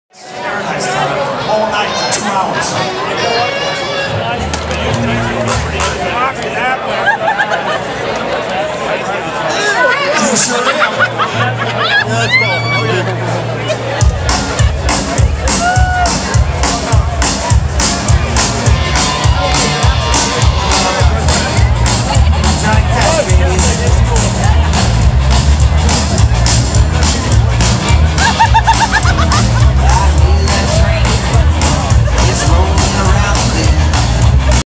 RAGBRAI 2018 - Plunger Shot
Some team had plungers to drink beer out of, this was my first time doing so.